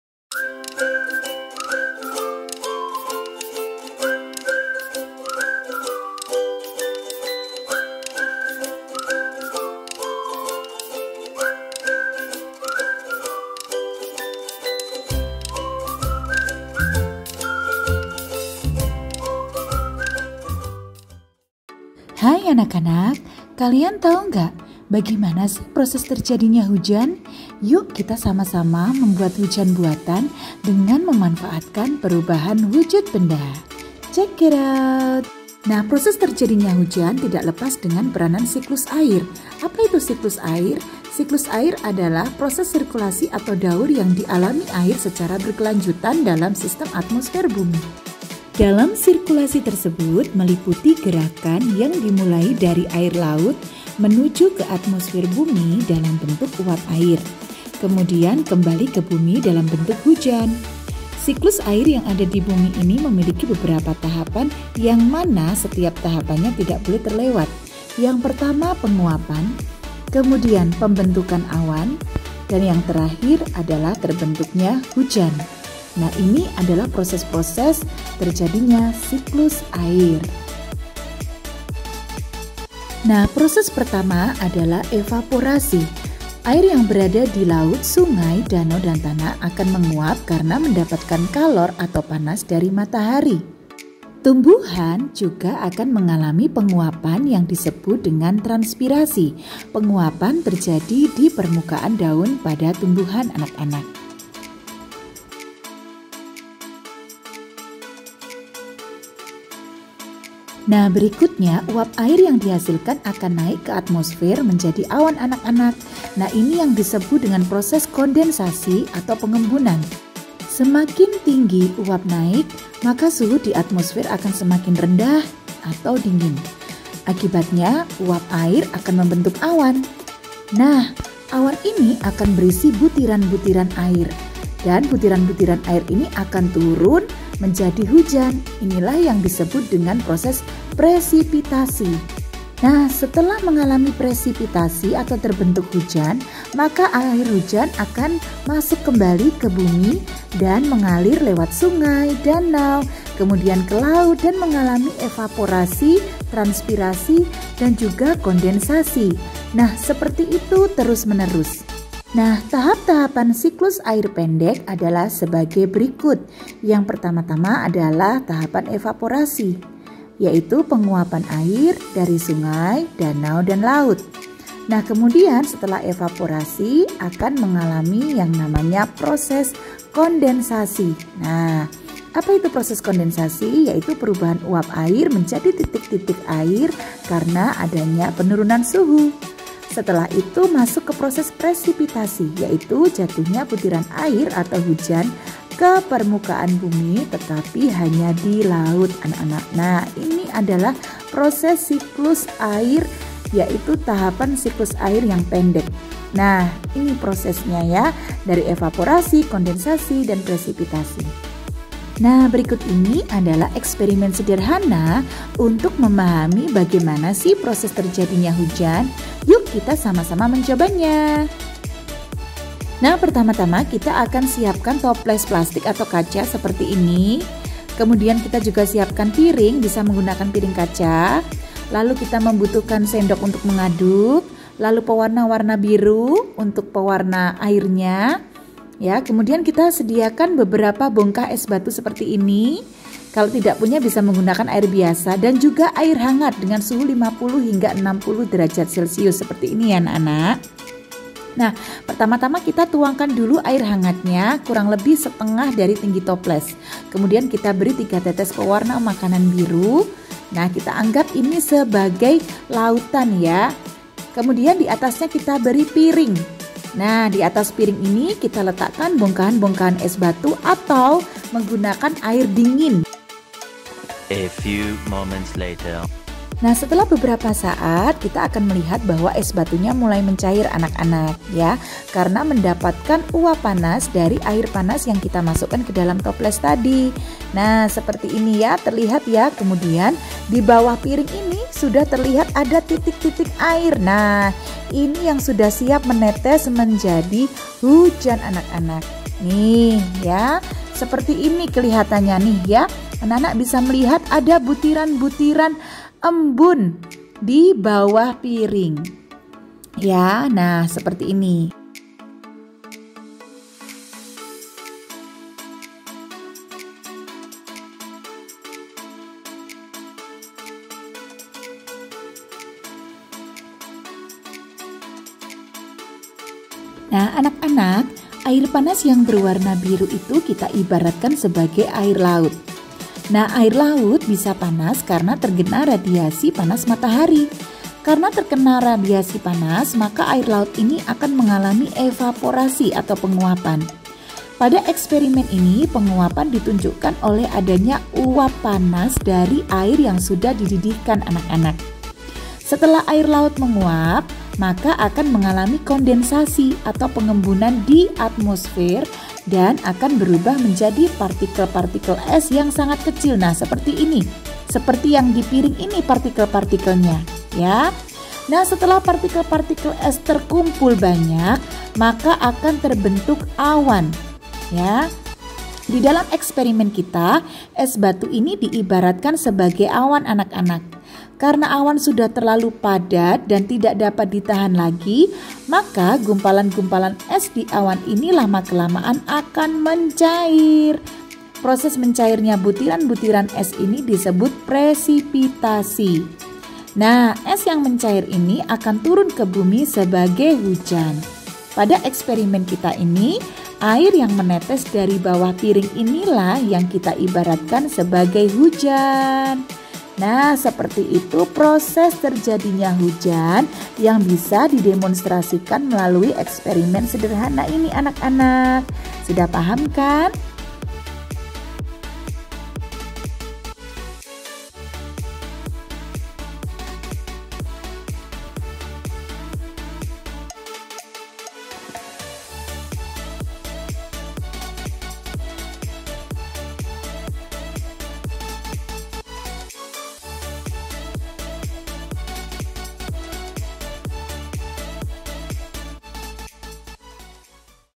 Science Experiment_4th Grade_Bagaimana Proses Terjadinya Hujan? (Perubahan Wujud)
Kegiatan Penilaian Sumatif Akhir